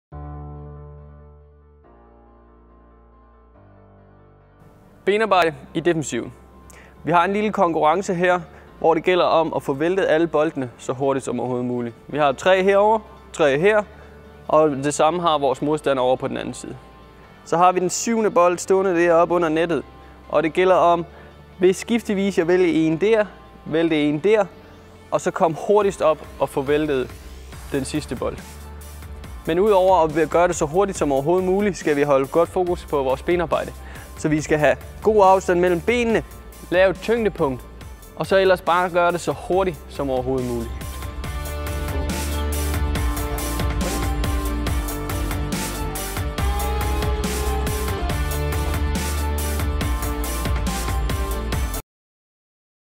Bevægelse – Hurtigt benarbejde på mellembanen
Der stilles 6 bolde op på hver sidelinje og en bold i midten under nettet. På tegn fra træneren bevæger spillerne sig fra side til side og vælter en bold af gangen. Det gælder om at være hurtigst og dermed nå at vælte bolden i midten under nettet.

Det er vigtigt, at spillerne husker at foretage et forspændingshop med efterfølgende acceleration ud mod siden. Der skal være fokus på, at der anvendes korrekt benarbejde. Spillerne skal desuden være opmærksomme på at trykke væk fra sidelinjen og tilbage mod midten.